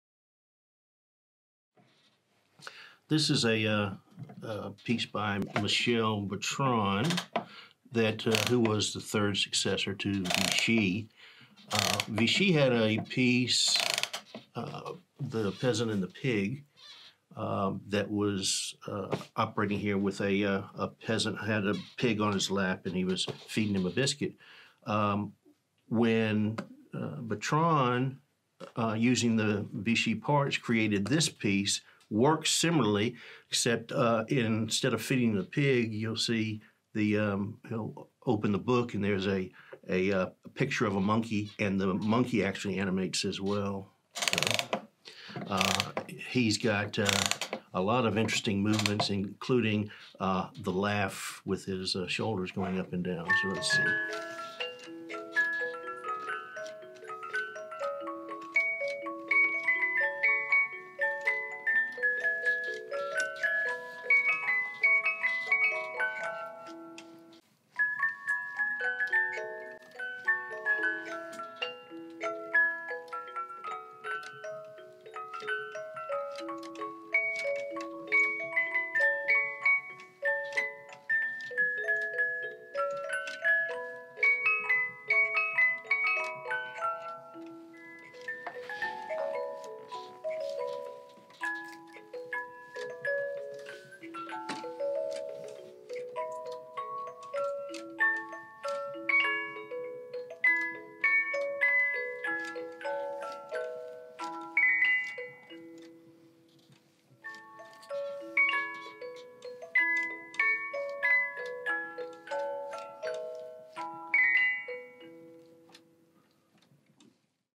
Lot 41: Ernest the Poet Musical Automaton
AUTOMATA: Life & Other Illusions • Public Auction Potter & Potter Auctions • Aug. 24, 2019 • Est. $7,000-9,000 View Lot

Switzerland: Michel Bertrand, ca. 1982. Sitting atop the back of a white wooden chair with a satchel of paintbrushes at one side, the poet holds a sketchbook in one hand and a pencil in the other. When the mechanism is activated, the automaton apparently draws further in the book, while moving his eyes, lips, and hands. Handsomely dressed in the bohemian style, including plaid pants, hat, yellow shirt with purple bow tie, and bearing a monocle in one eye. Composition head and hands with glass eyes. Height 36”. Modeled on the Victorian design by Vichy.